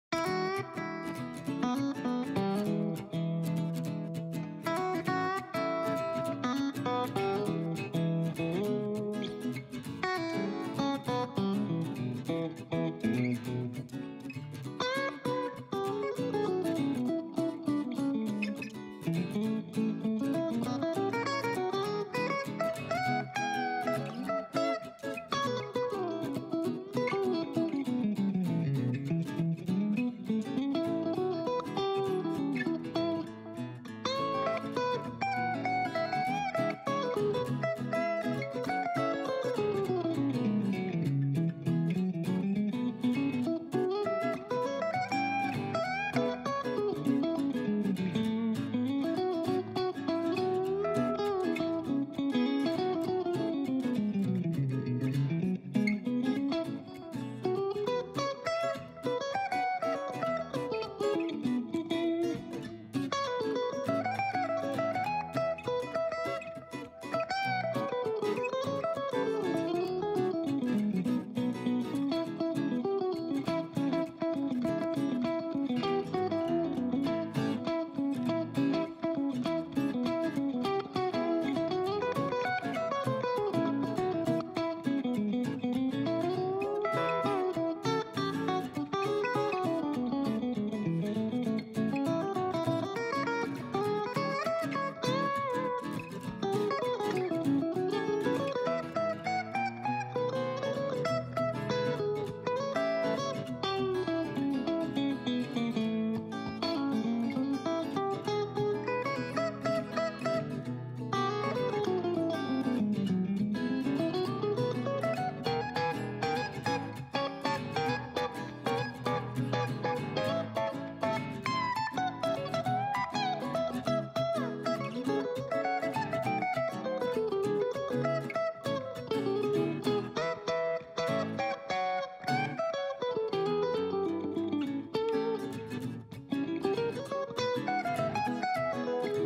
[4K , Ai vlog] How do you like my shirt? 제 셔츠 어때요? | shirt Lookbook AI LOOKBOOK
AI art model created with Stable Diffusion.

In this concept art, I made ai art of Pink shirt and underwear  concept.

Stable Diffusion으로 만든 AI아트 모델입니다.

이번 컨셉아트는 핑크 셔츠와 언더웨이 컨셉으로 AI아트를 만들어 보았습니다.
즐거운 감상 되시기 바랍니다.